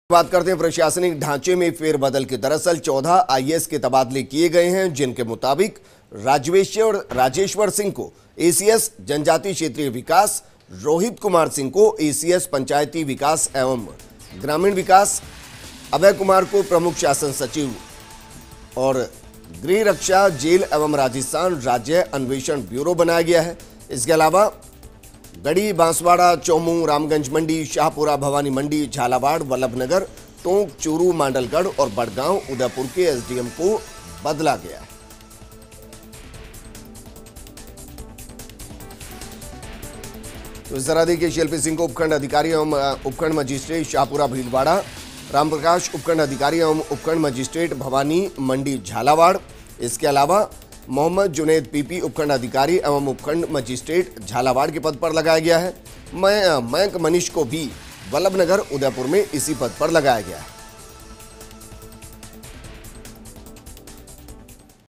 Rajasthan में 14 IAS अफसर तबादलों का पोस्टमार्टम !
About this Video:
Post mortem of 14 IAS officer transfers in Rajasthan!